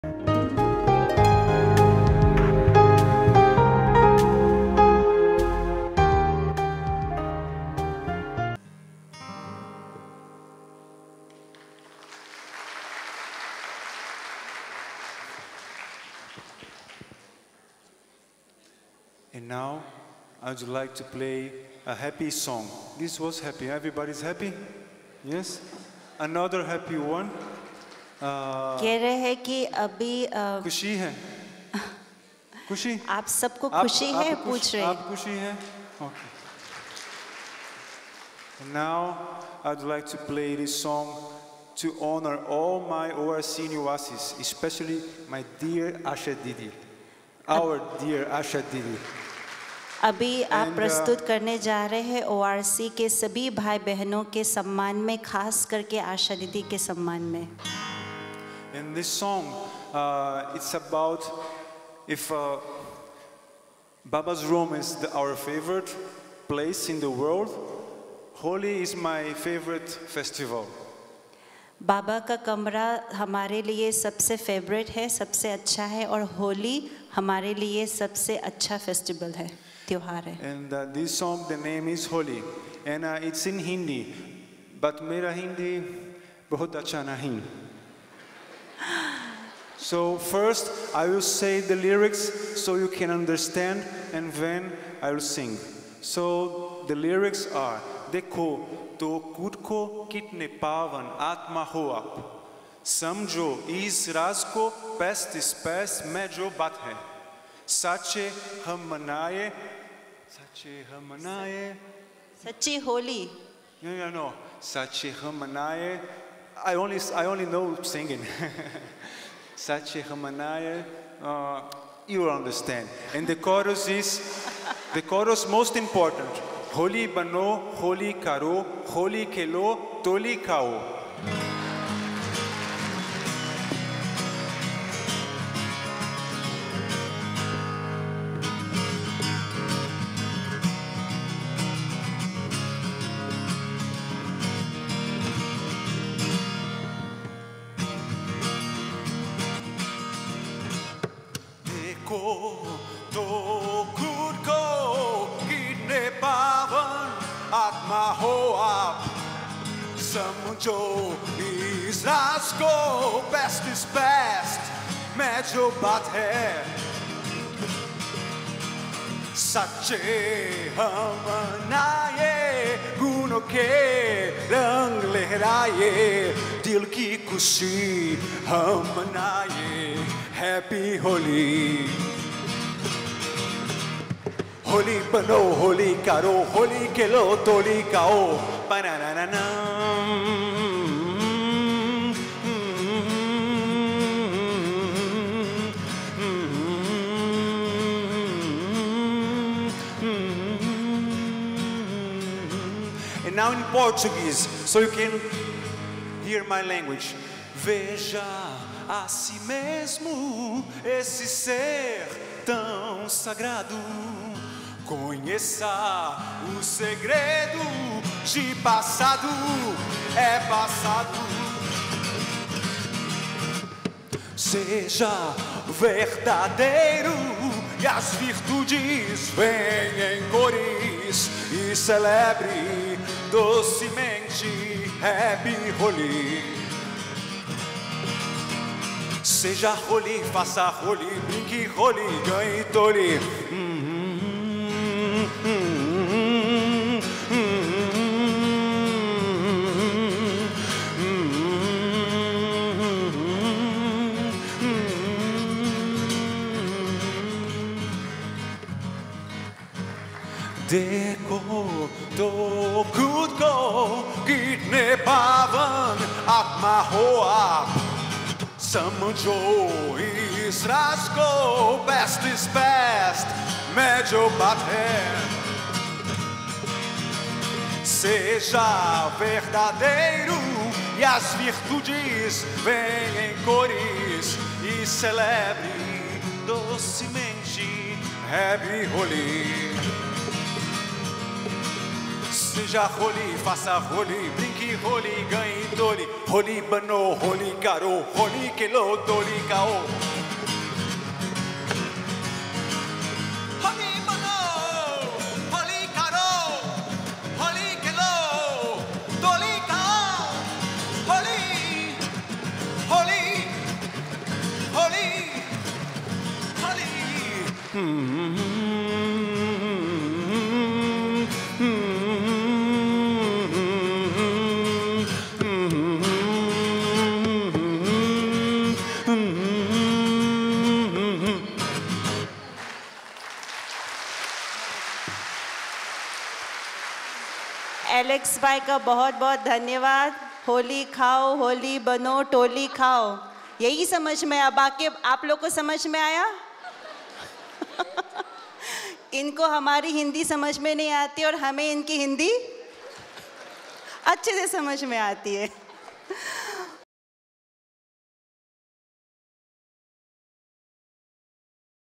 Holi Song BK Alex, Brazil
Holi Song BK Alex, Brazil  during Cultural Program at Om Shanti Retreat Centre